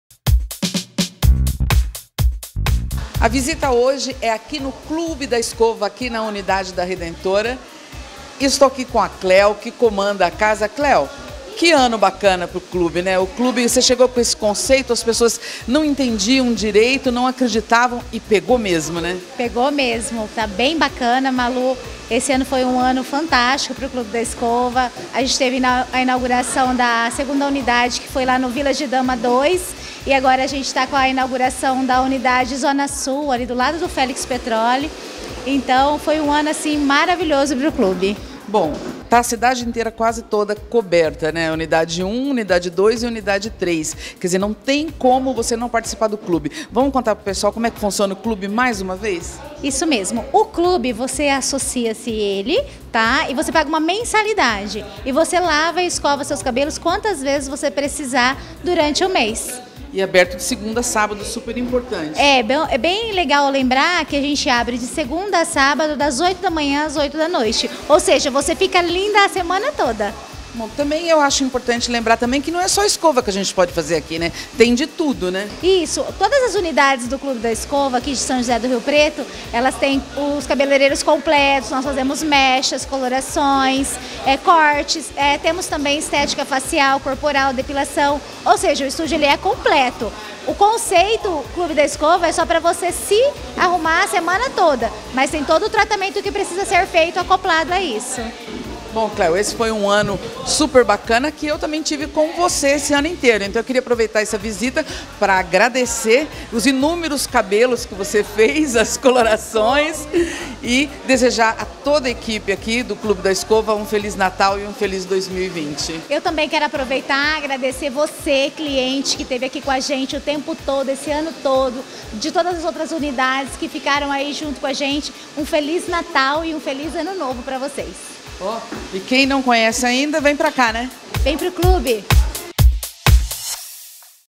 Visita no Clube da Escova
O Clube da Escova que tem no comando a profissional Cléo Lopes tem muito a comemorar neste fim de ano. O Clube encerra 2019 com três unidades. Acompanhe!